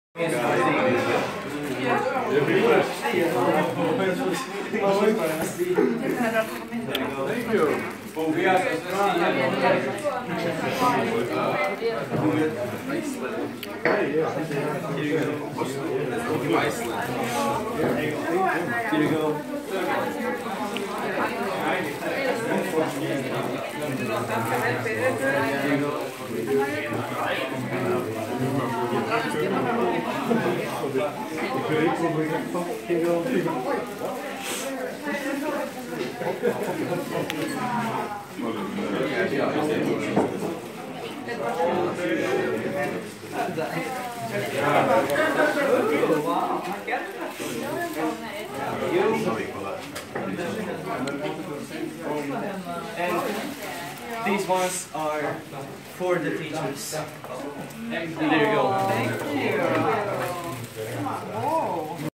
Marko dijeli Islanđanima suvenire koje je sam izradio#Erasmus+F2FCult#Thematic art workshop#GGTŠS
Marko is delivering his hand made souvenirs to his Icelandic peers.